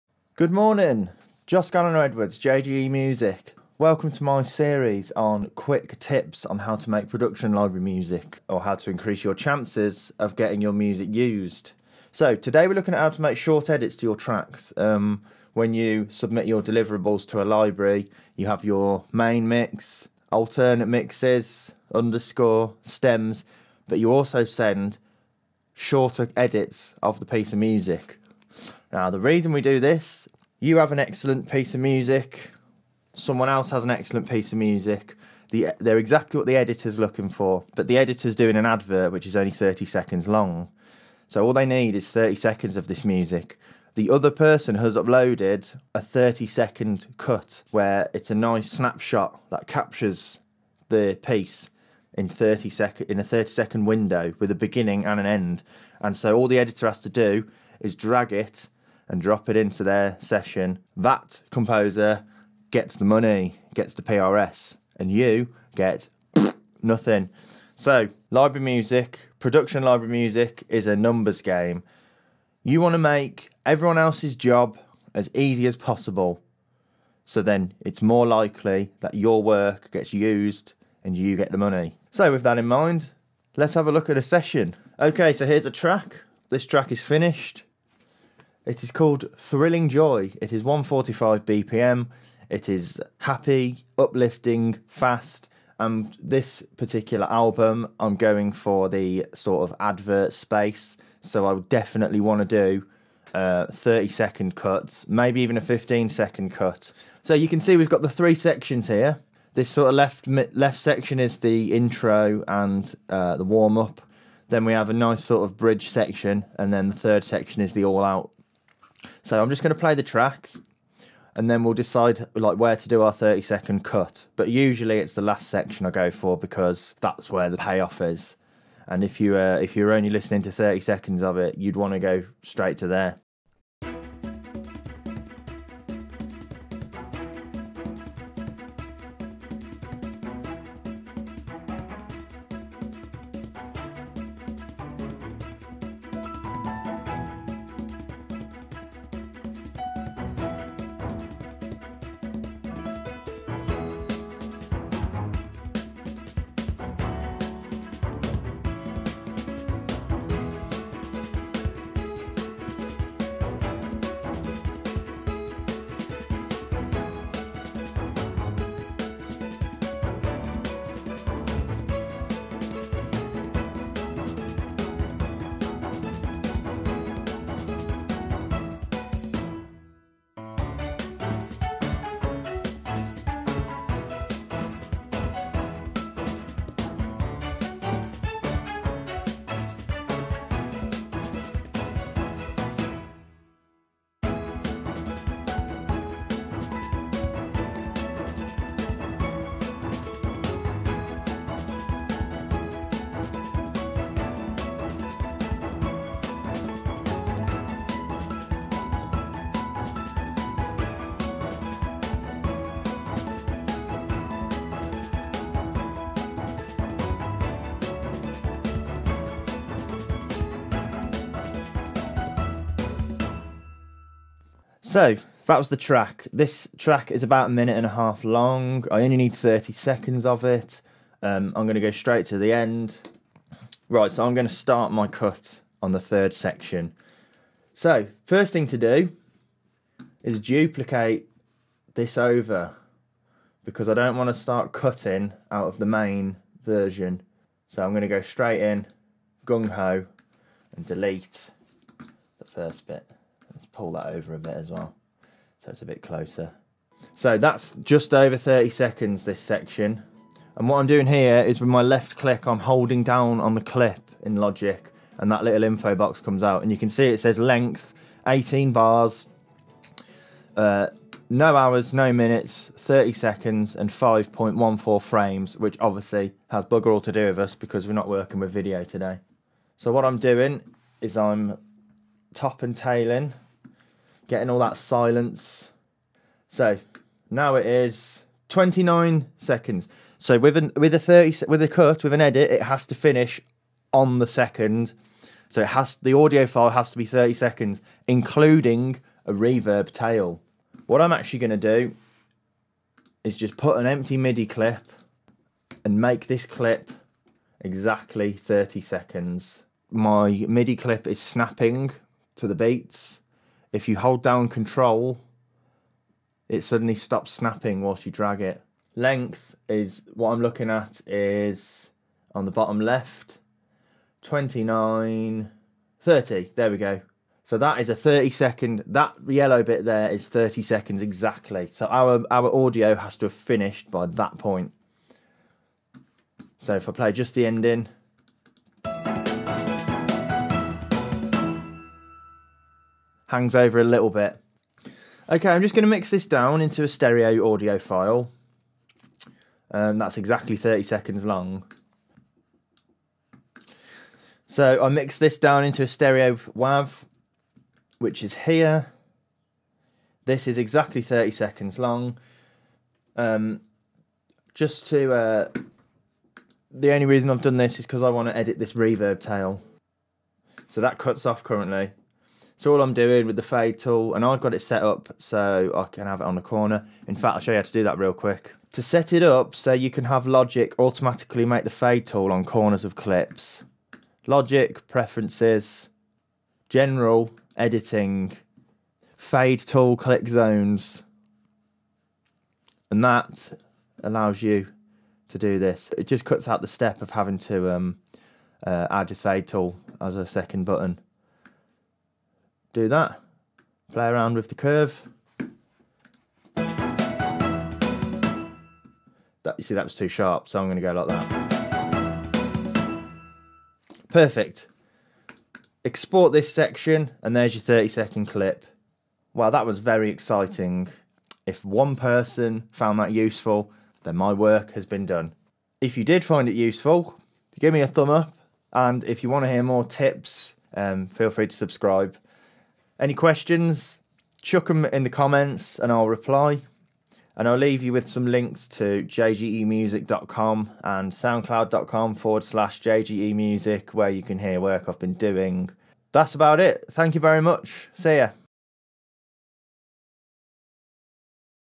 Production Library Music Tips: How to create short edits (15, 30, 60 seconds) JGE MUSIC
How to create Production Library Music: The Basics (old video)

Composed, performed and recorded by Joss Gallanagh-Edwards

Chapters:

0:00 - Intro
0:28 - Why we need short edits
1:25 - Track overview
2:13 - Listening to the track
3:45 - Track finish and deciding where to cut
4:04 - Editing MIDI regions
5:45 - Editing reverb tail
6:14 - Fade tool tip
6:59 - Outro

©Joss Gallanagh-Edwards 2021